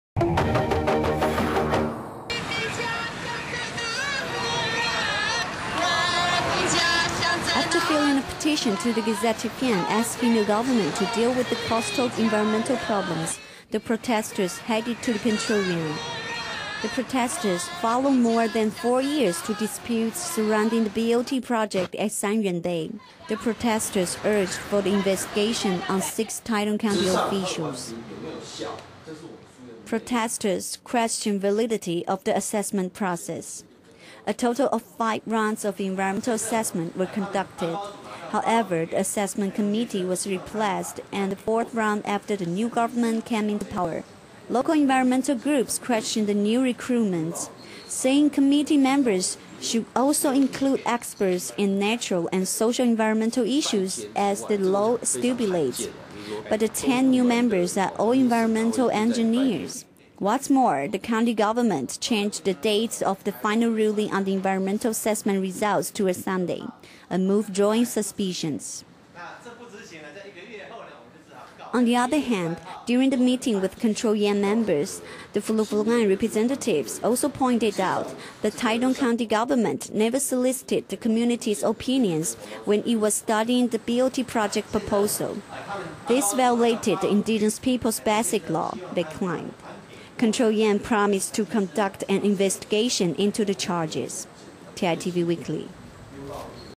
0921 #85 Petition Against Resort Project
After submitting the petition to the Executive Yuan, protesters headed to the Control Yuan. They handed a petition asking the Control Yuan to investigate on Taitung County Magistrate 鄺麗貞, the countys Environmental Protection Bureau Director 黃明恩 and four other officials for what they called collaboration with entrepreneurs. Separately, the Fulafulangan people also asked for a probe into the violation of the Indigenous Peoples Basic Lawover the BOT project. The Control Yuan promised to carry out the investigations.